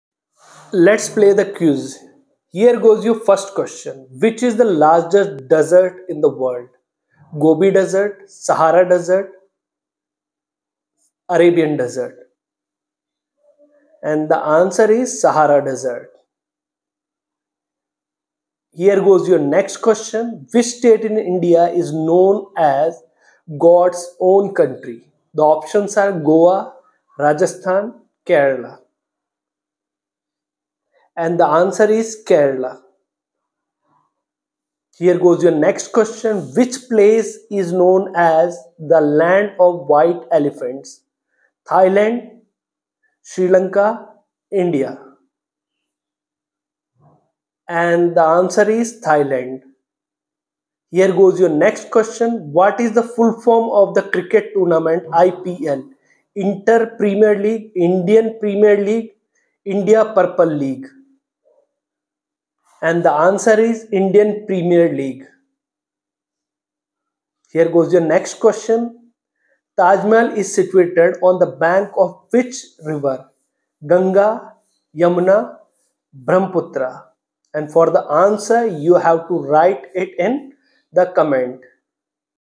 Quiz 26
1) Which is the largest desert in the world?
   a) Gobi Desert
   b) Sahara Desert
   c) Arabian Desert

**Answer:**
   b) Sahara Desert

2) Which state in India is known as "God's Own Country"?
   a) Goa
   b) Kerala
   c) Rajasthan

**Answer:**
   b) Kerala

3) Which place is known as the "Land of White Elephants"?
   a) India
   b) Thailand
   c) Sri Lanka

**Answer:**
   b) Thailand

4) What is the full form of the cricket tournament IPL?
   a) Inter Premier League
   b) Indian Premier League
   c) Indiana Premier League